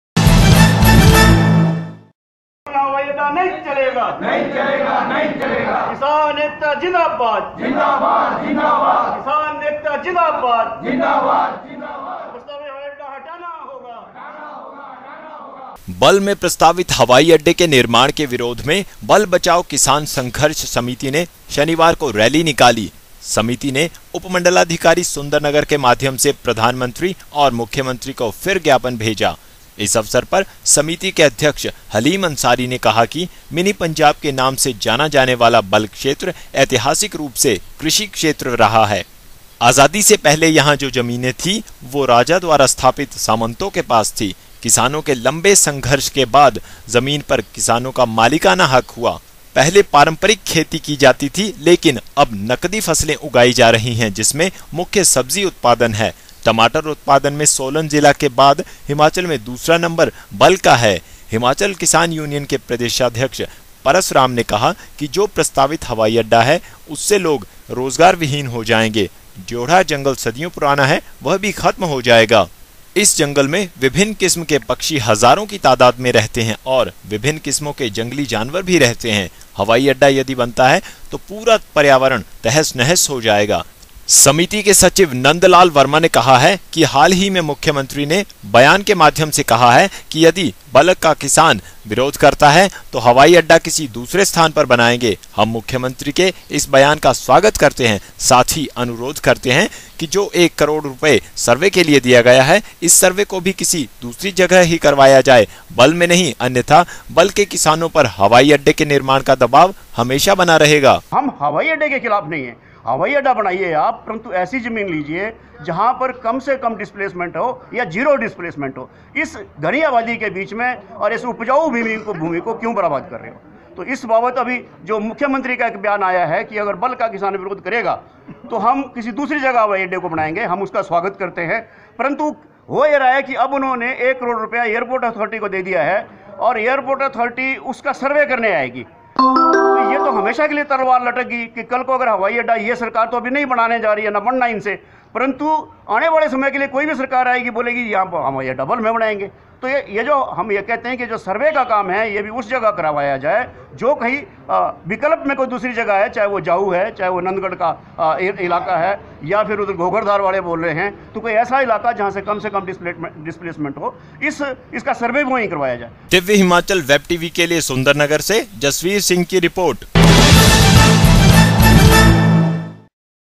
बल्ह में प्रस्तावित हवाई अड्डे के विरोध में सड़कों पर उतरी बल्ह बचाओ किसान संघर्ष समिति।
बल्ह में प्रस्तावित हवाई अड्डे के विरोध में सड़कों पर उतरी बल्ह बचाओ किसान संघर्ष समिति। दिव्य हिमाचल टीवी